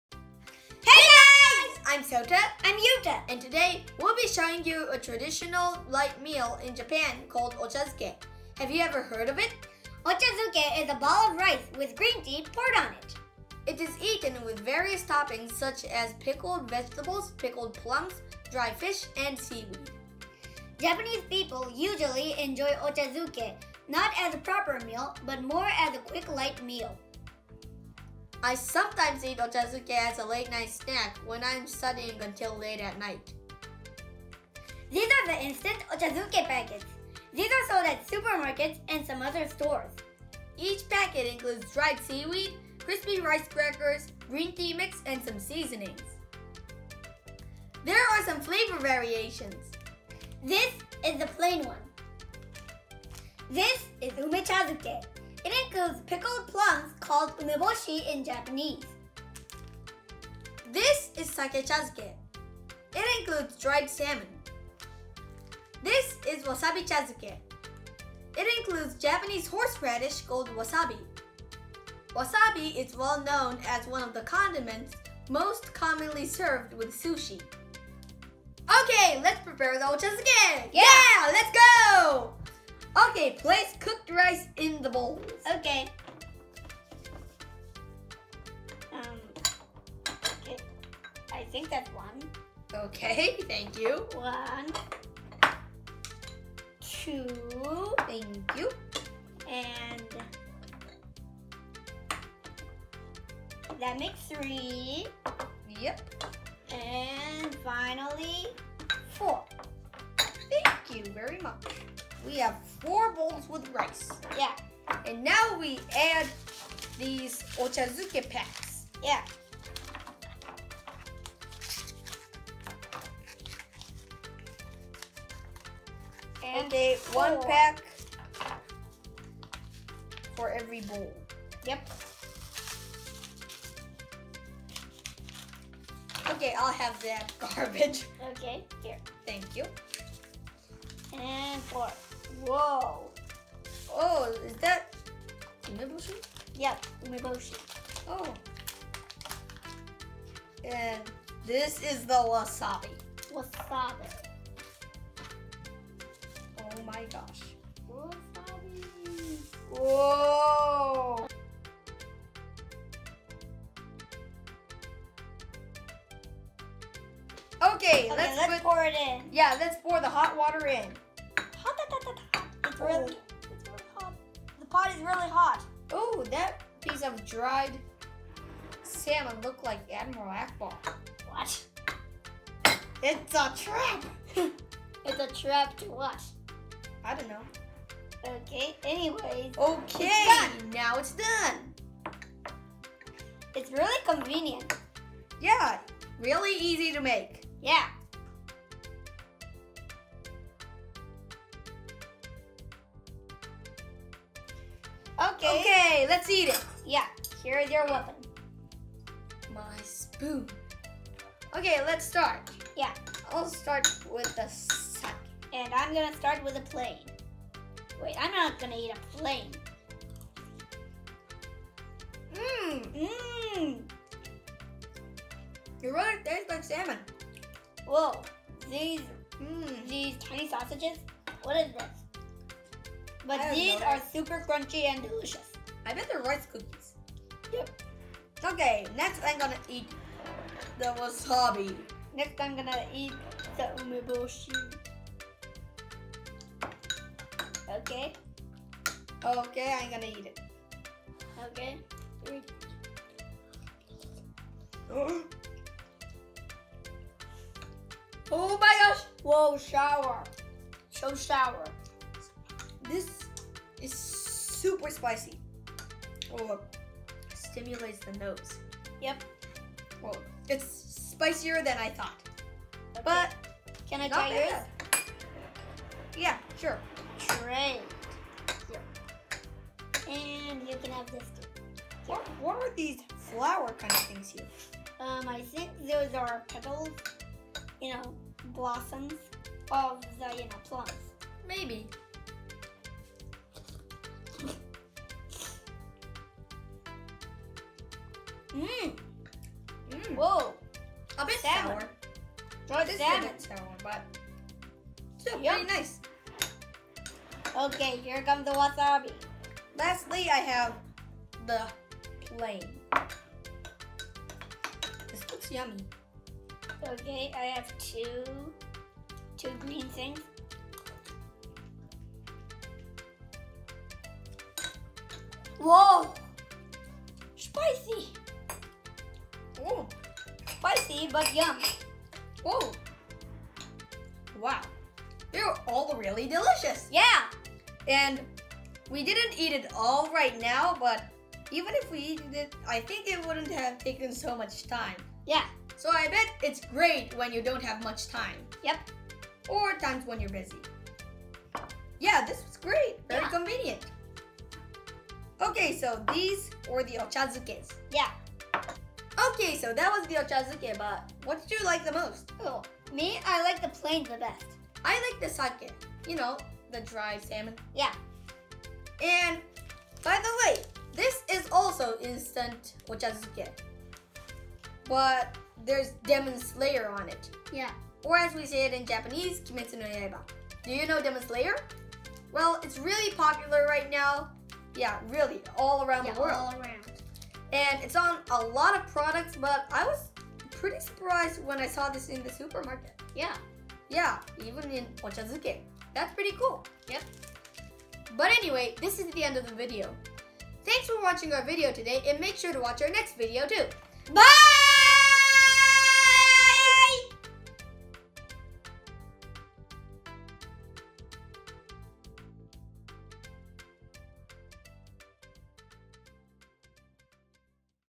What "Ochazuke" is Like / Instant Ochazuke Packet / Japanese Quick Meal
These are instant ochazuke packets❗️
Watch till the end to see the "Demon Slayer (Kimetsu-no-Yaiba)" instant ochazuke packet😄

We introduce many things about Japan🇯🇵 to people around the world.